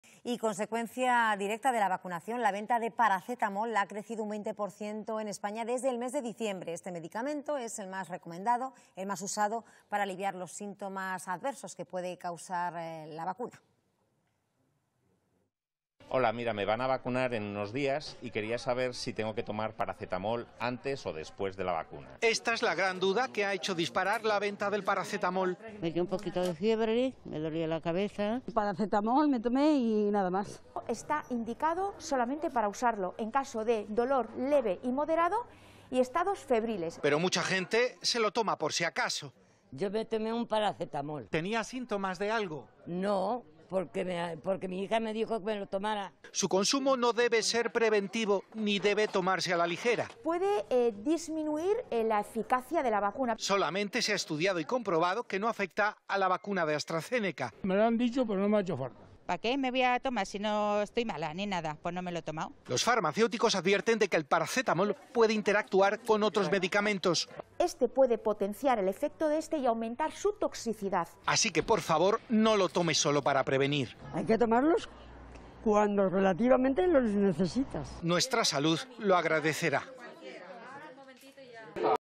6/05/2021 Rosalía Gozalo advierte de los peligros que puede conllevar el uso del paracetamol
Durante la campaña de vacunación contra la COVID-19 se han incrementado las ventas de paracetamol en las farmacias.

Rosalia Gozalo advierte de los peligros que pueden conllevar automedicarse con este fármaco cuando no existen síntomas antes o despues de recibir la vacuna